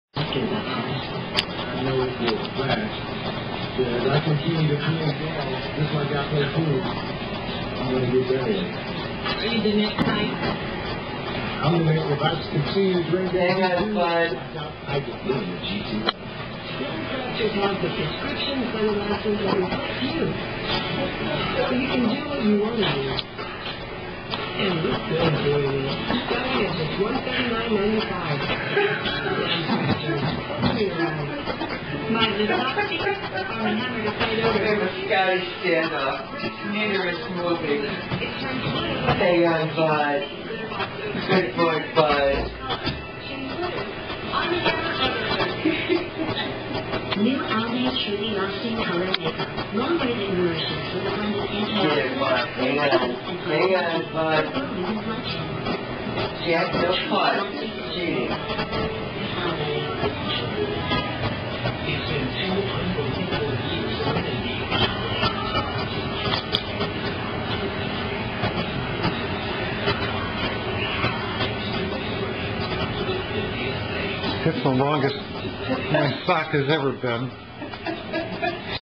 The longest sock
Scotties playing tug-o-war